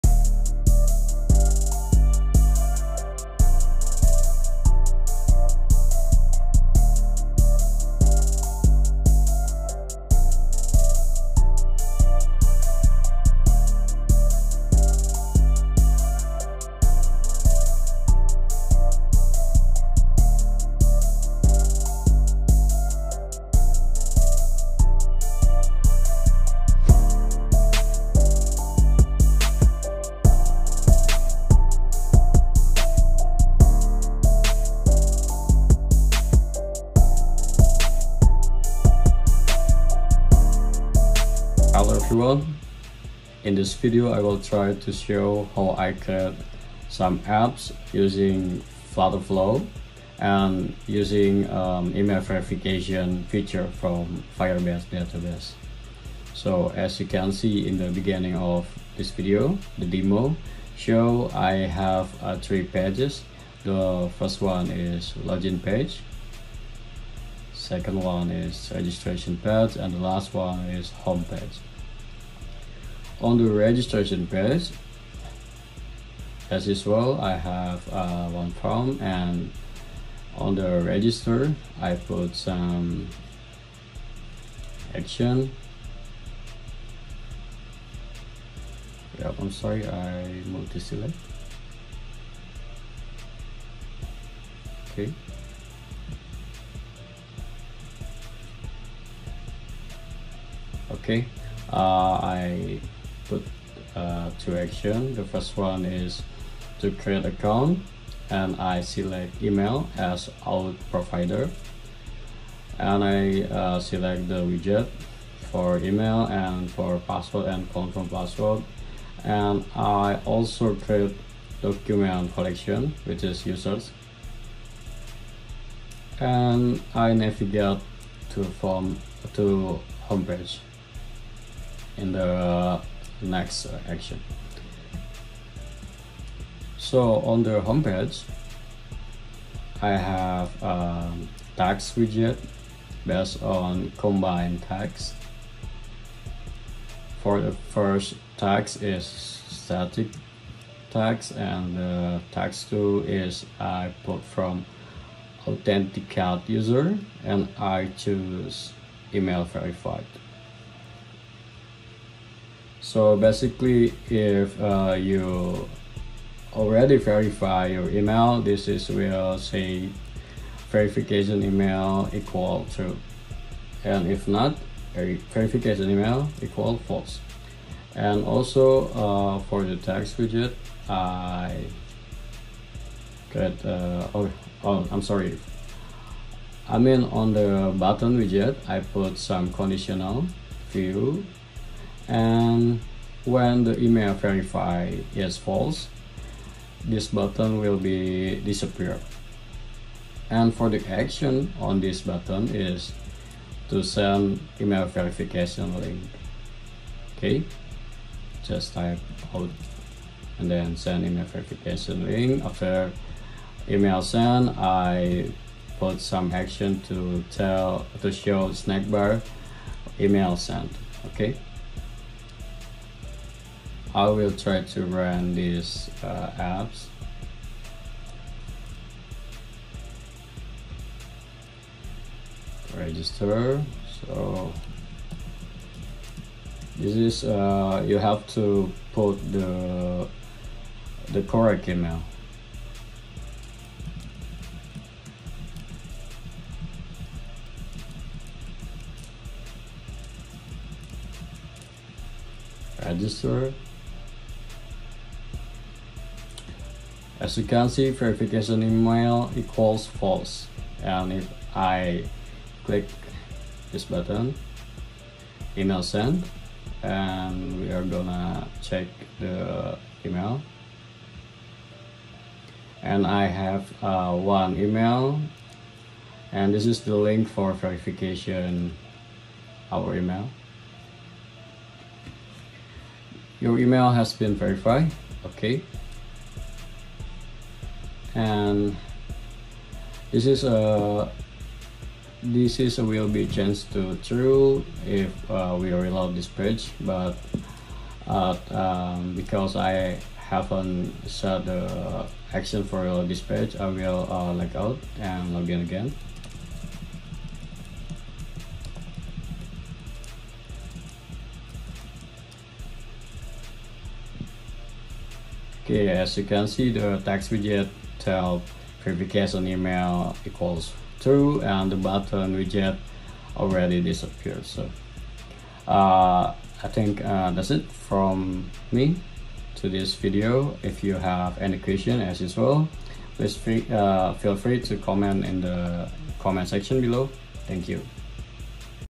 Flutterflow using Firebase authentication email verification
In this video i’m gonna show how i create flutterflow app using features email verification from firebase.
If you have offering jobs/apps to me, sent me an email to